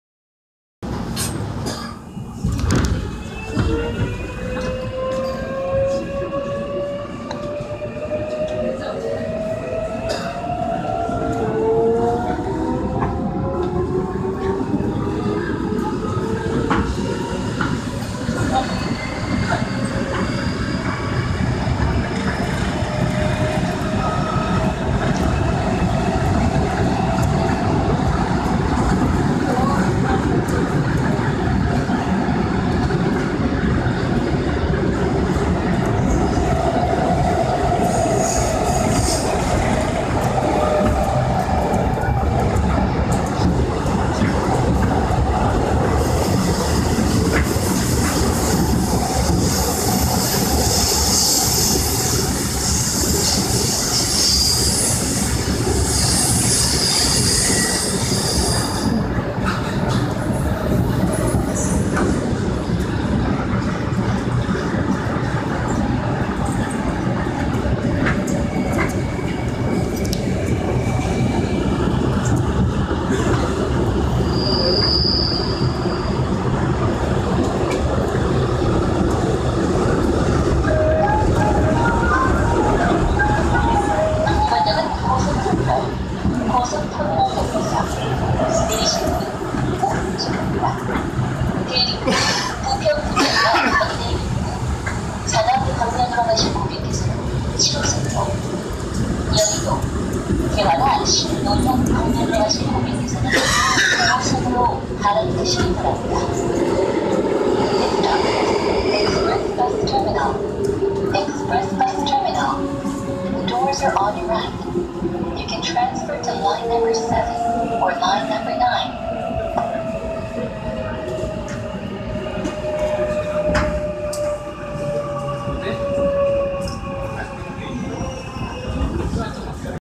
코레일 3호선 (KORAIL, 韓国鉄道公社3号線3070系) 교대역 ~ 고속터미널역 주행영상
374편성
교대역 ~ 고속터미널역

Seoul Subway Line 3 Seoul National Univ. of Education ~ Express Bus Terminal